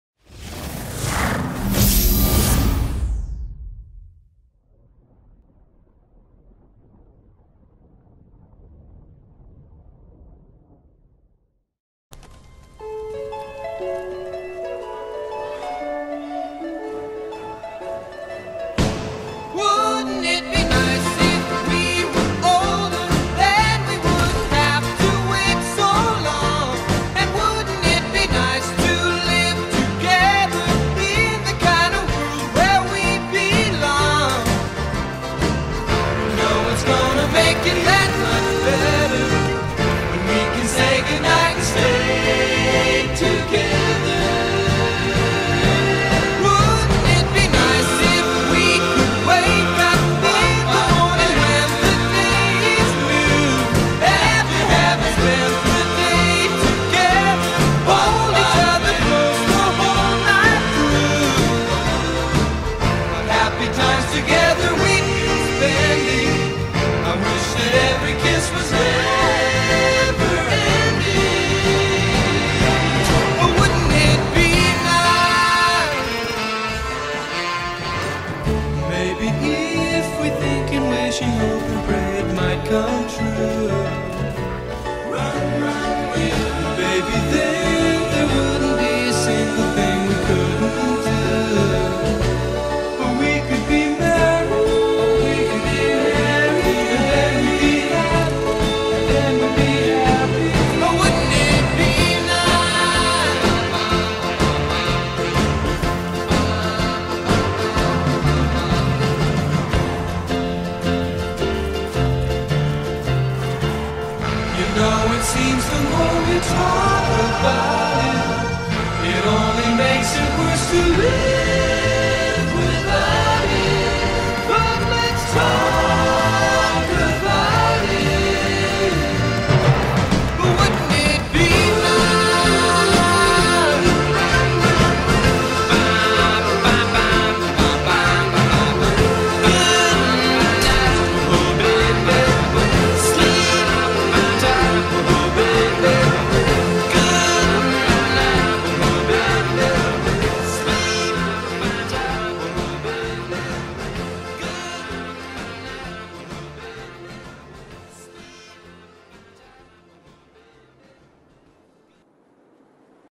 Channel Trailer
This Is A New One Because The Old One Is Outdated. Also If You Think This Isn't A Trailer Cause This Didn't Tell You What It Was About, It Showed All The Game I Upload To The Channel. (AKA All The Games I Have On The Playstation)